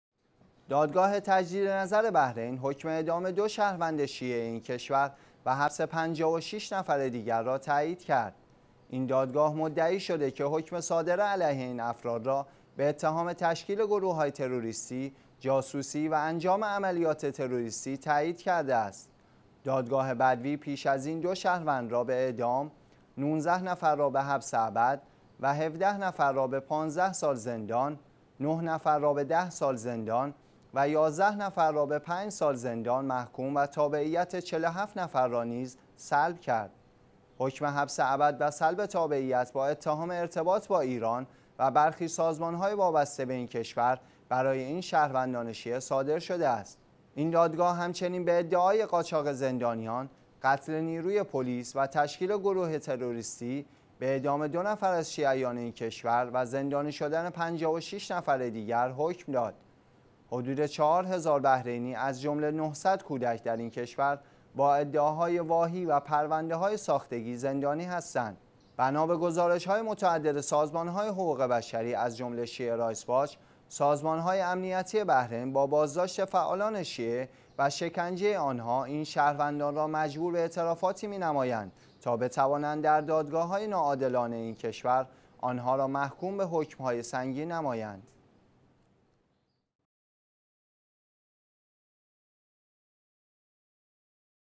تائید حکم اعدام 2 شیعه بحرینی در دادگاه بحرین و سلب تابعیت 47 نفر
دادگاه تجدیدنظر بحرین حکم اعدام دو شهروند شیعه این کشور و حبس 56 نفر دیگر را تأیید کرد.
این دادگاه مدعی شده که حکم صادره علیه این افراد را به اتهام تشکیل گروه های تروریستی، جاسوسی و انجام عملیات تروریستی تایید کرده است.
دادگاه بدوی پیش از این دو شهروند را به اعدام، 19 نفر را به حبس ابد و 17 نفر را به پانزده سال زندان، 9 نفر را به ده سال زندان و 11 نفر را به پنج سال زندان محکوم و تابعیت 47 نفر را نیز سلب کرد.
حکم حبس ابد و سلب تابعیت با اتهام ارتباط با ایران و برخی سازمان های وابسته به این کشور برای این شهروندان شیعه صادر شده است.
این دادگاه همچنین به ادعای قاچاق زندانیان، قتل نیروی پلیس و تشکیل گروهی تروریستی، به اعدام دو نفر از شیعیان این کشور و زندانی شدن ۵۶ نفر دیگر حکم داد.
حدود چهارهزار بحرینی از جمله 900 کودک در این کشور با ادعاهای واهی و پرونده های ساختگی زندانی هستند.
بنابه گزارش های متعدد سازمان های حقوق بشری از جمله شیعه رایتس واچ، سازمان های امنیتی بحرین با بازداشت فعالان شیعه و شکنجه آنها، این شهروندان را مجبور به اعترافاتی می نمایند تا بتوانند در دادگاه های ناعادلانه این کشور، آنها را محکوم به حکم های سنگین نمایند.